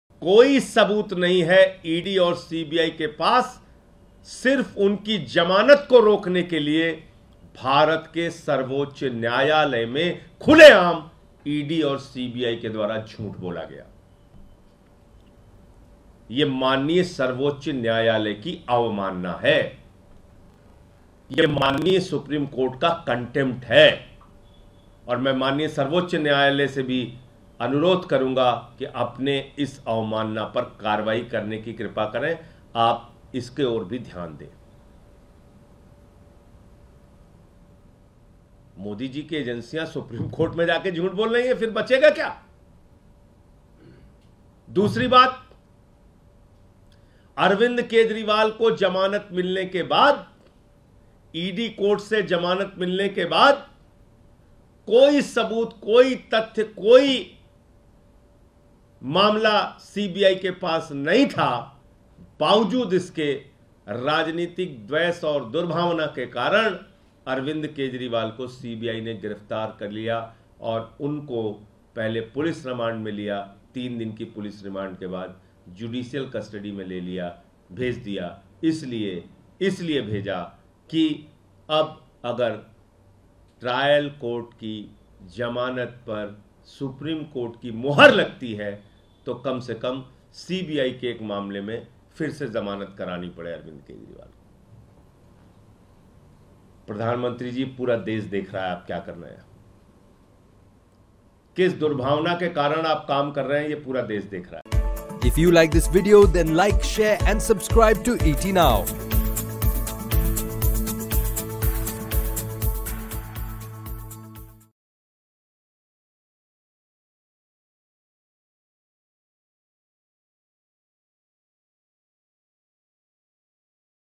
Sanjay Singh Slams ED and CBI Over Arvind Kejriwal's Arrest: Alleges Contempt of Court | Top News
AAP Rajya Sabha MP and leader Sanjay Singh launched an attack on the ED and the CBI over the arrest of Arvind Kejriwal by the CBI after a Delhi court gave bail to Delhi’s CM in ED case. Sanjay Singh accused the investigating agencies of Contempt of Court.

Delhi CM Arvind Kejriwal Remanded to 14 Days Judicial Custody in Liquorgate Probe | CBI | Top News